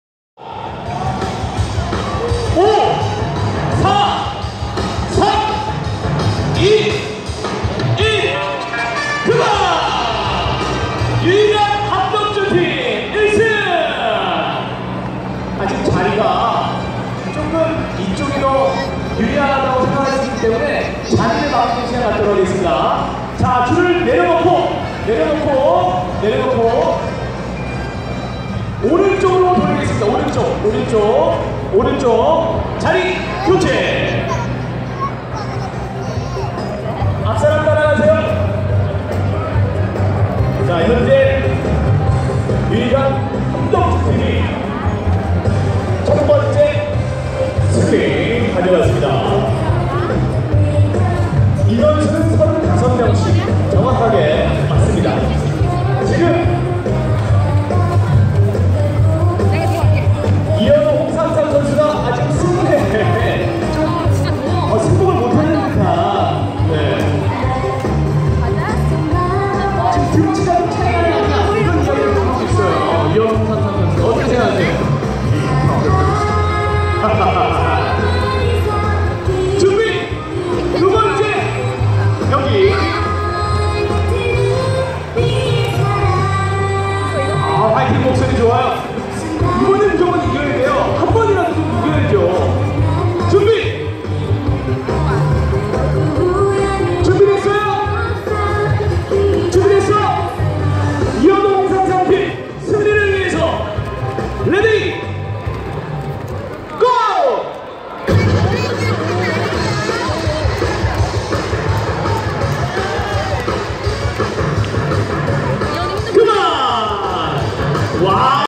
170505 두산 베어스 vs LG 트윈스 어린이날 줄다리기
170505 두산 베어스 vs LG 트윈스 어린이날 줄다리기